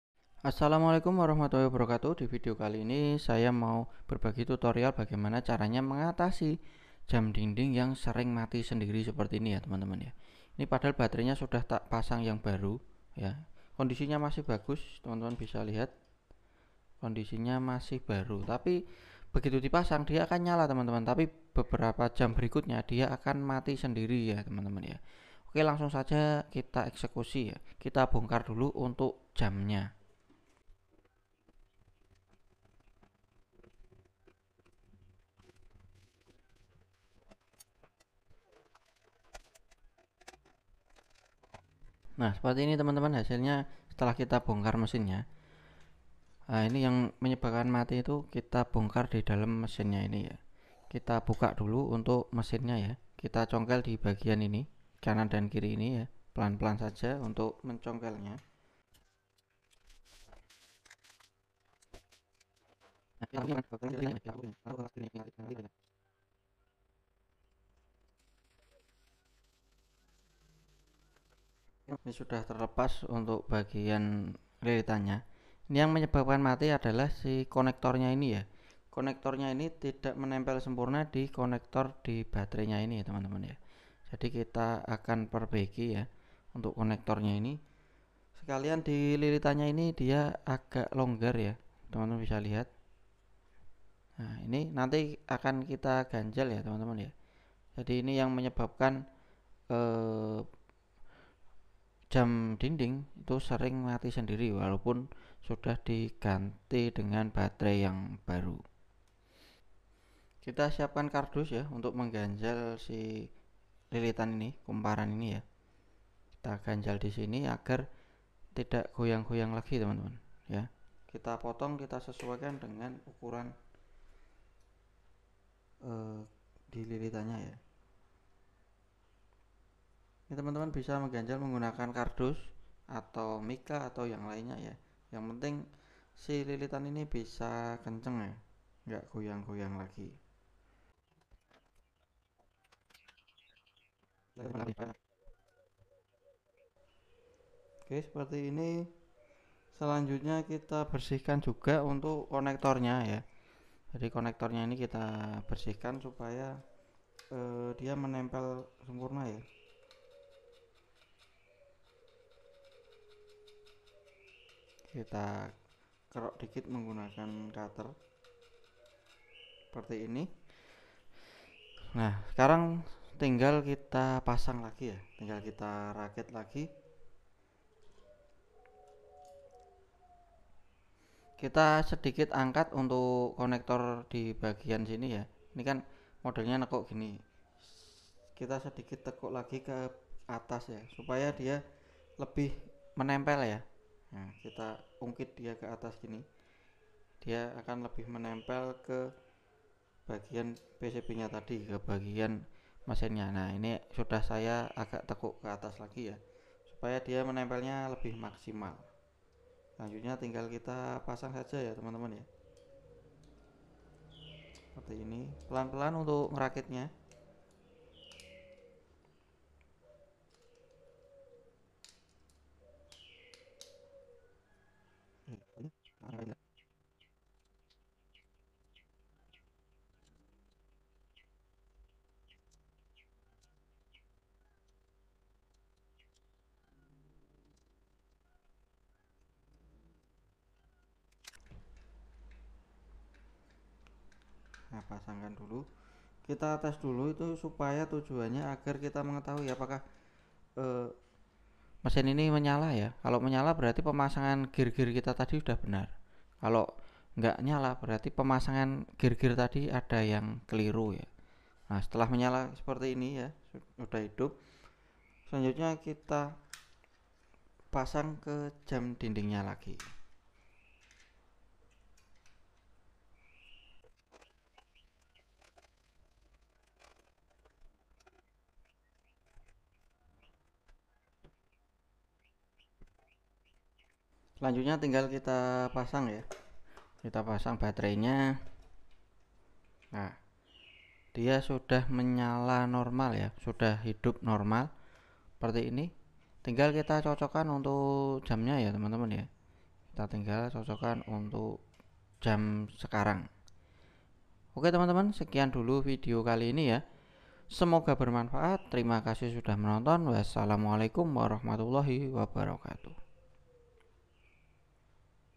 CARA MEMPERBAIKI JAM DINDING YANG SERING MATI SENDIRI‼️
cara mudah memperbaiki jam dinding yang sering mati sendiri walaupun sudah diganti dengan baterai baru.